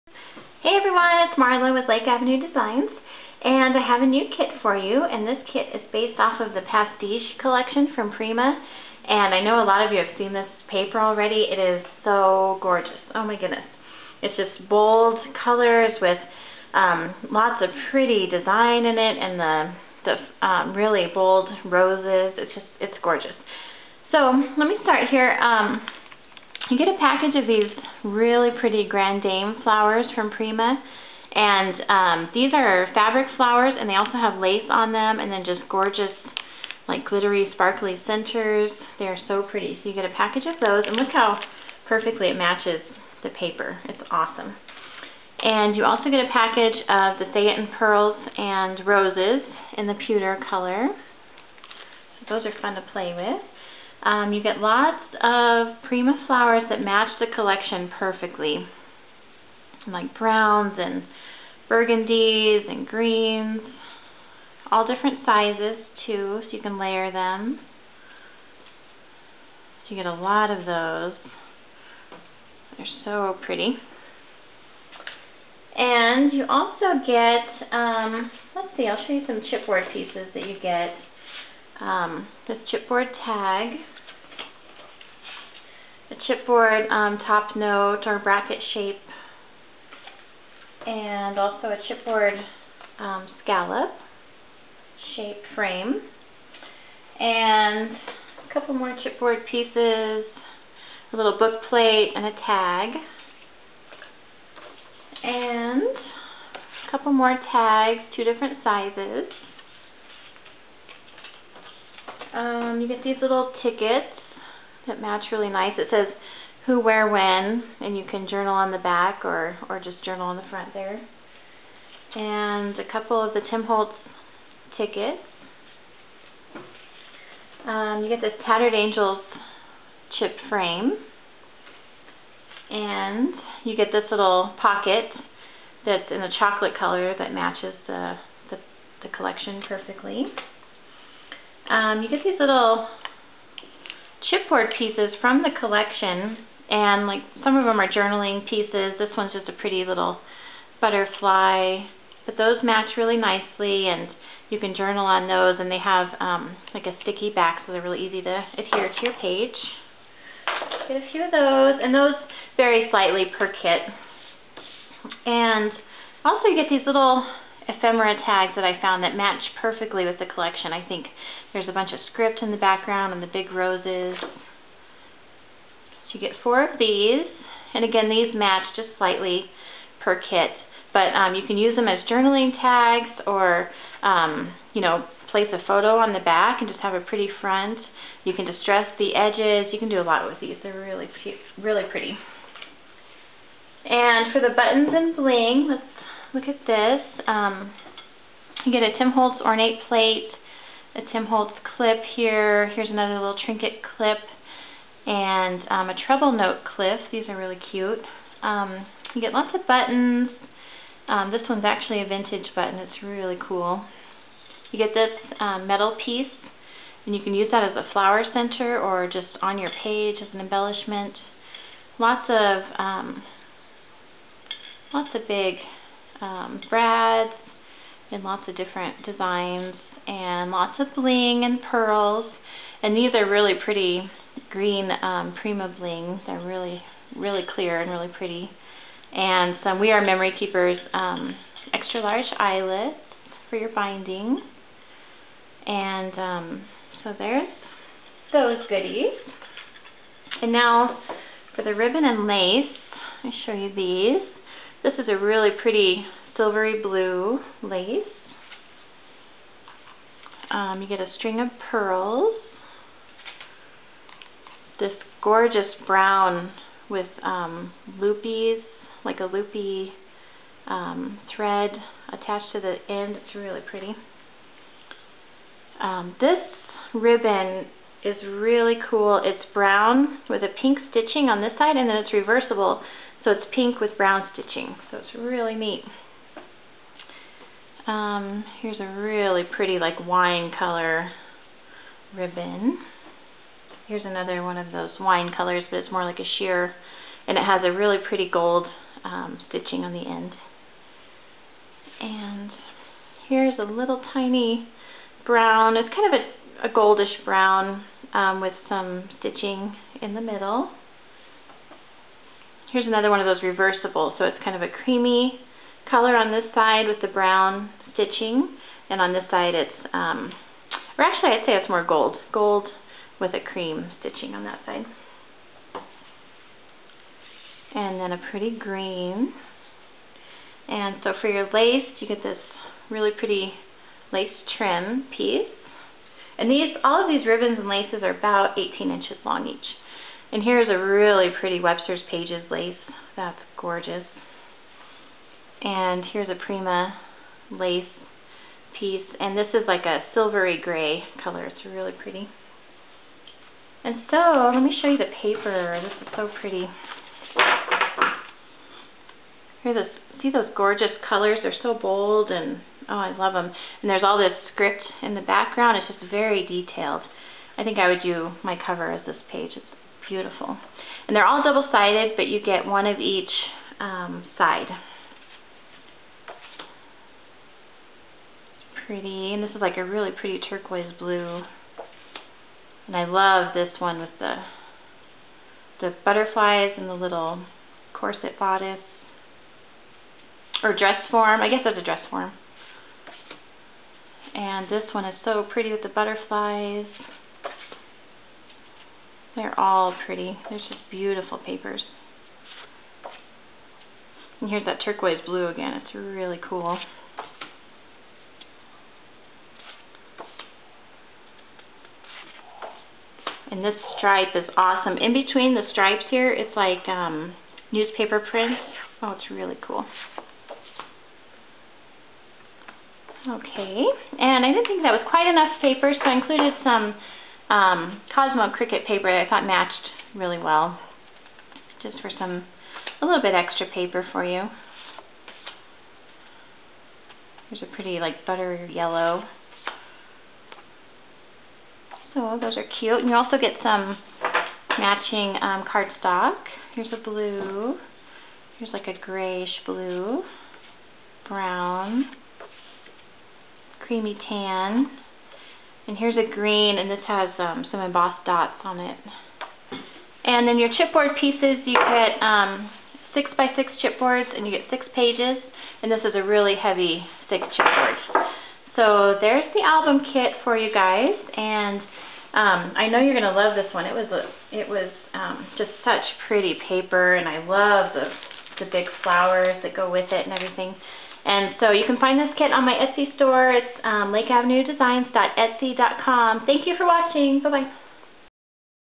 Pastiche Mini Album Kit - Scrapbooking Kit
This is a mini album kit using the Pastiche paper line from Prima.